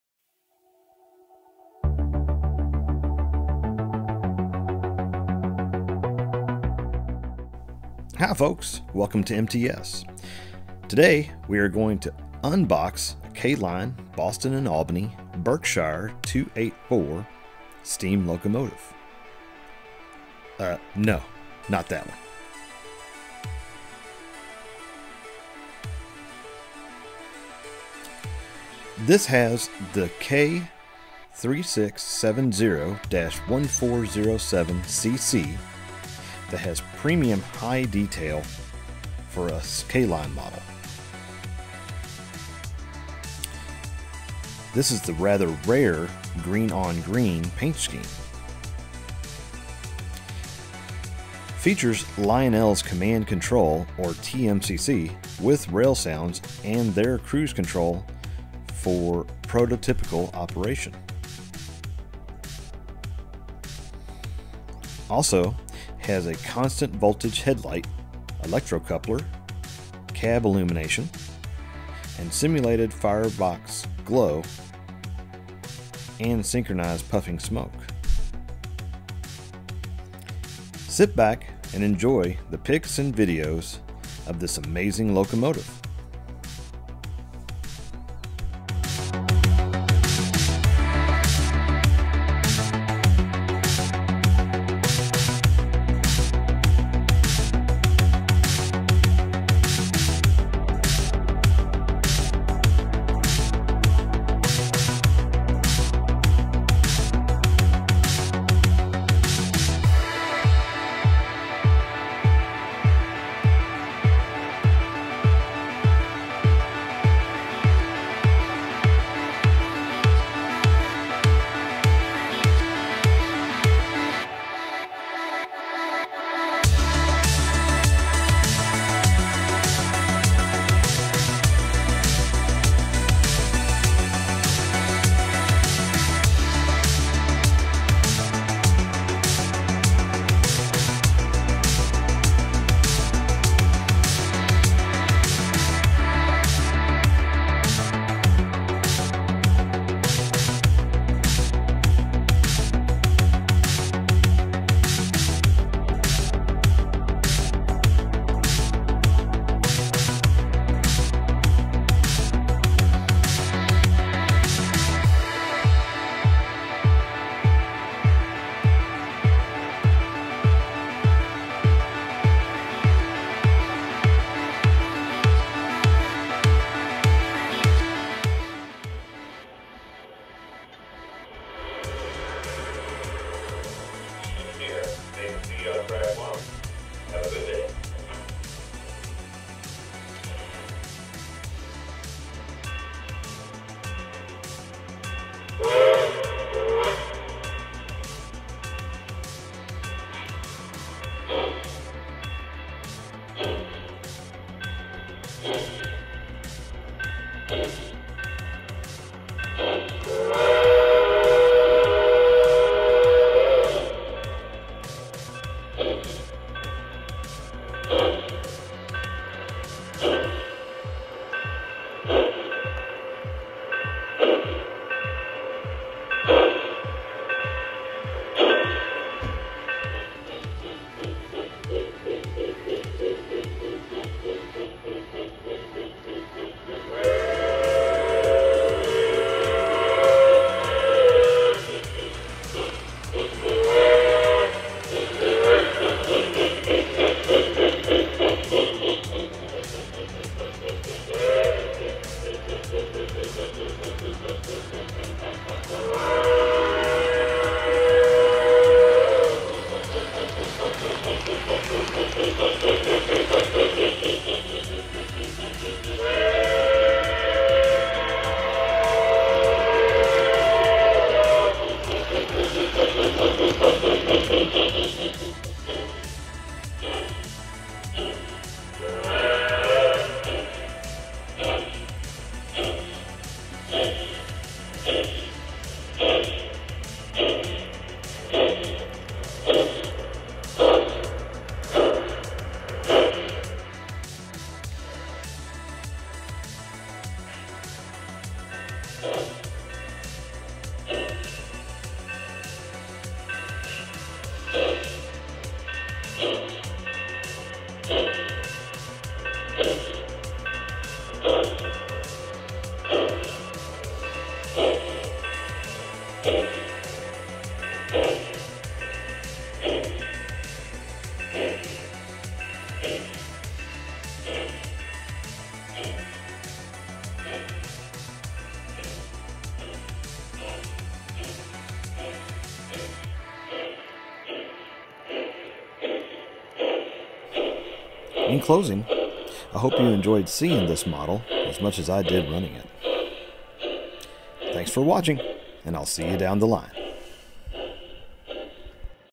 Unbox and Operating K-Line B&A Berkshire 2-8-4 Steam Locomotive
Welcome to MTS.  Today we are going to unbox and operate a K-Line Boston and Albany Berkshire 2-8-4 steam locomotive. This is the K3670–1407CC that features Lionel’s command control with railsounds and cruise control. Sit back and enjoy the pics and videos of this amazing locomotive.